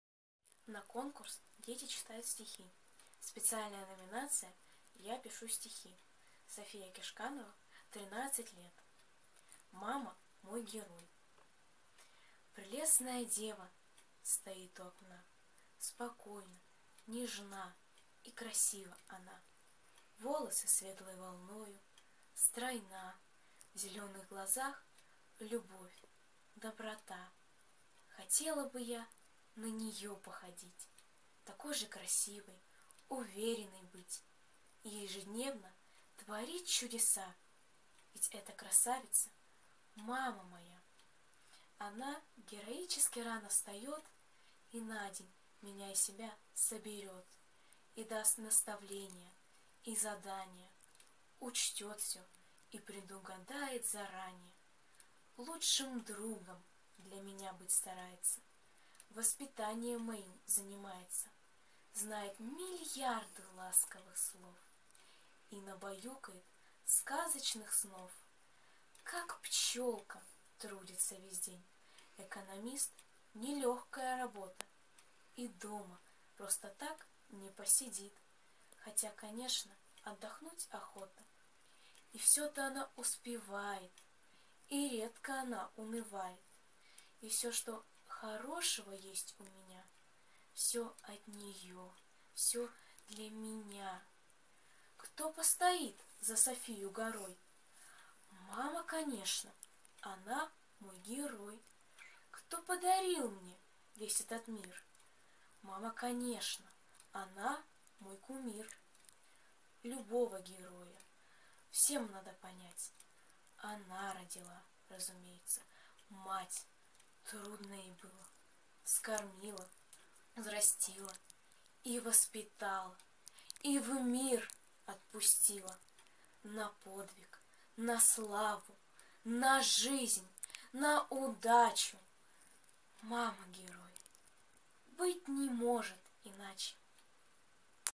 София Кишканова «Мама- мой Герой!»
Очень люблю свою маму и поэтому решила посвятить ей свое стихотворение. Я занимаюсь литературой:  Пишу рассказы,сказки и стихи. Цените и любите своих мам,они у нас единственные и неповторимые! Надеюсь,что вам понравится мое стихотворение. Спасибо за внимание! с уважением, София Кишканова, город Новосибирск.